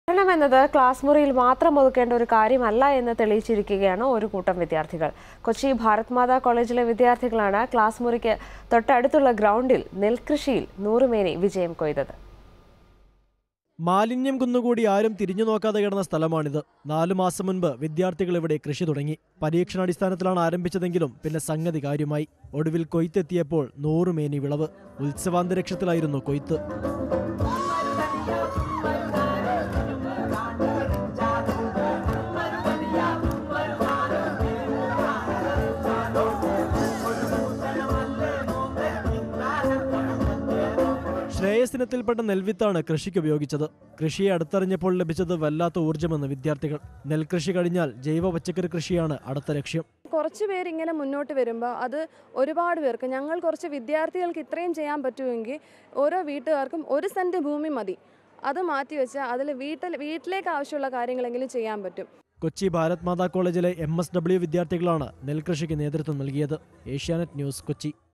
Bharata Matha College students success story of organic farming
students turns farmer . Kochi Bharata Matha College students success story of organic farming